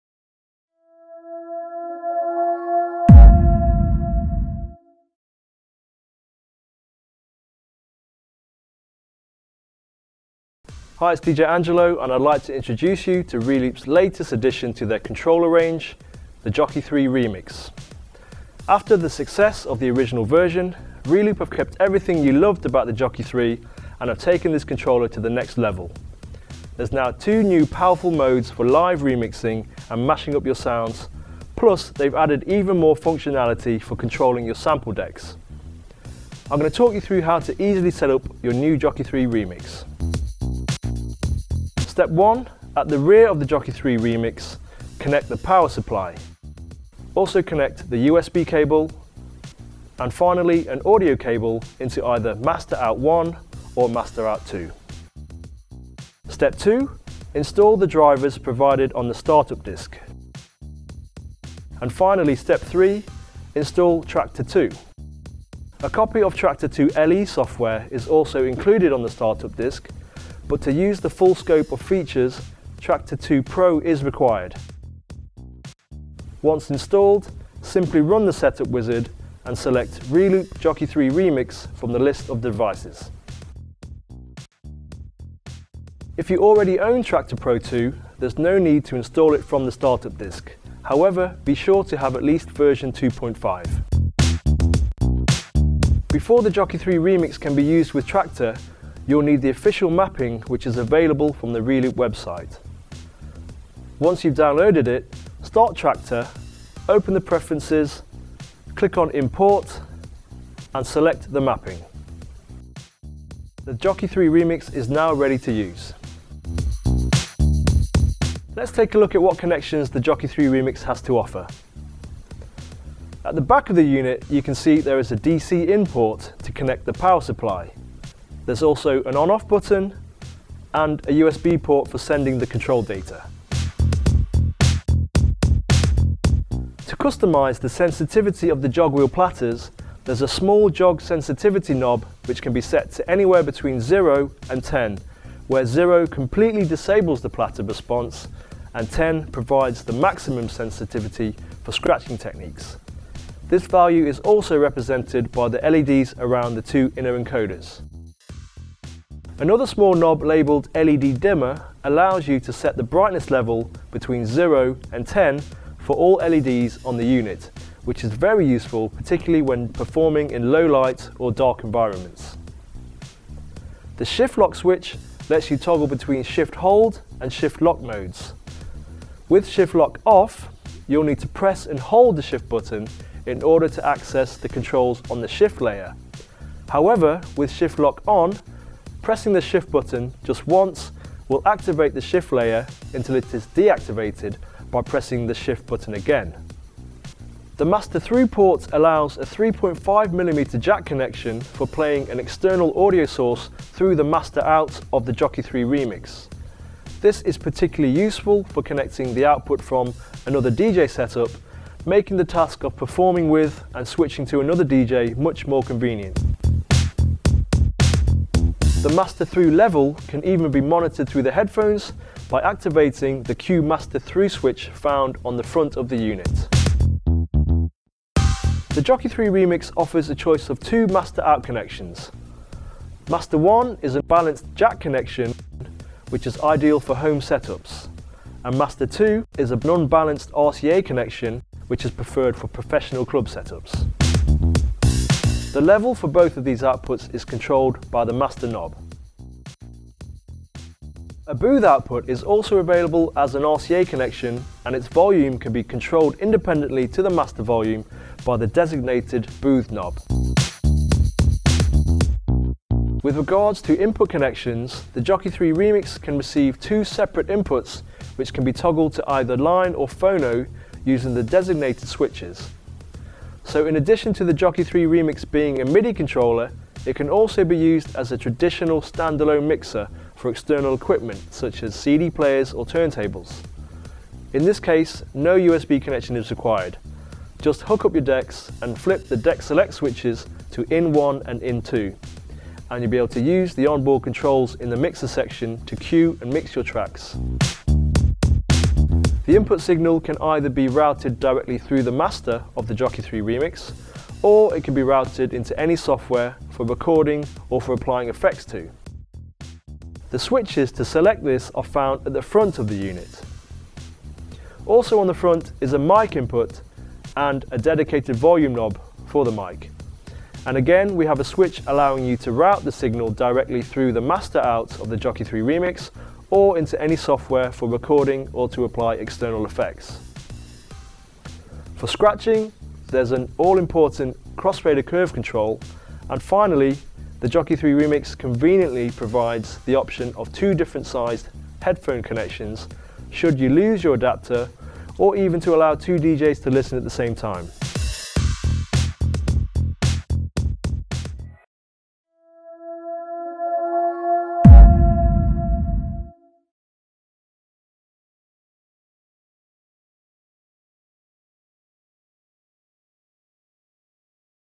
Reloop Jockey 3 Remix - Introduction (1/5)
Welcome to the Reloop Jockey 3 Remix - the new Traktor controller flagship!

Thanks to its powerful live control system, the Jockey 3 Remix is the ideal solution for advanced Traktor Pro users who won't compromise when it comes to live remixing, mashups, or spectacular FX manipulation, but are looking for a device that perfectly supports their professional and creative mixing skills. With its sturdy, full metal construction and just like its predecessor, the Jockey 3 Remix features premium processing characteristics, and thanks to the high-end 24 bit/96 kHz BurrBrown AD/DA converters, it makes the all the difference regarding sound quality for live applications.

Watch our NEW Reloop KUT Trailer - 2 Channel Battle FX DJ Mixer With innoFADER (Official Introduction)